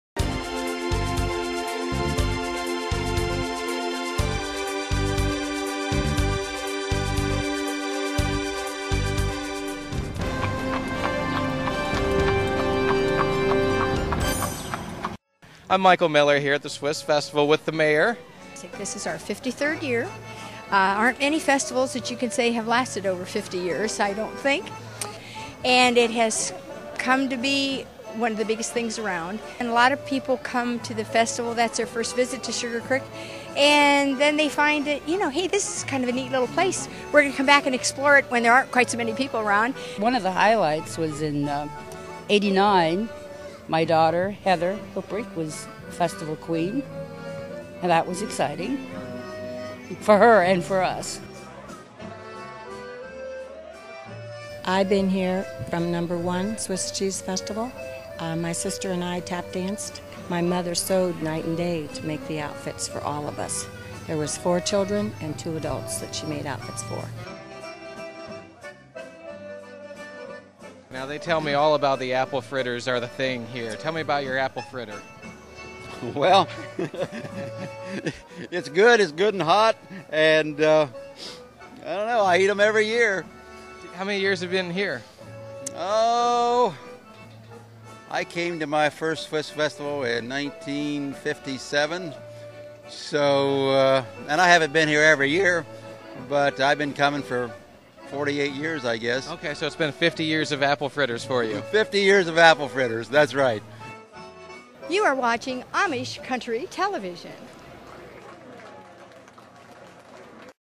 Ohio Swiss Festival, Sugarcreek, Ohio
| The Ohio Swiss Festival is fun for the whole family.  The village of Sugarcreek, Ohio comes alive every fall with 2 days of Food, Fun, Contests, Dancing, and a Grand Parade.  This year's Festival will be held September 28 & 29, 2007